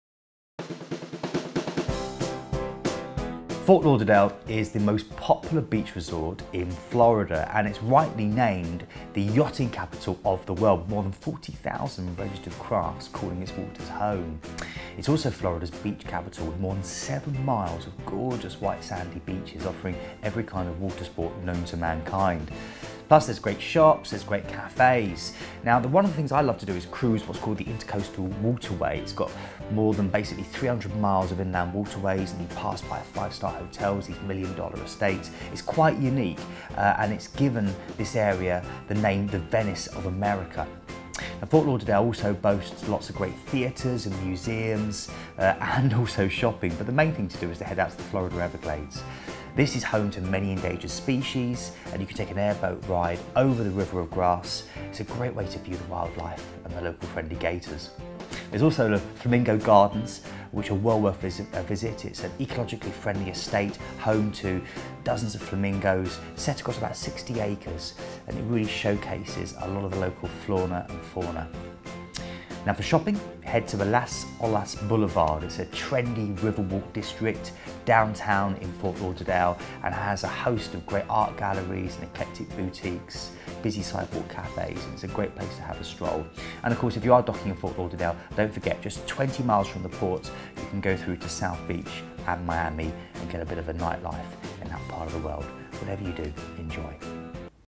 Keith's Top Tips - Fort Lauderdale | Planet Cruise
Our Cruise & Destination Expert Keith Maynard gives you his Top Tip's for Fort Lauderdale.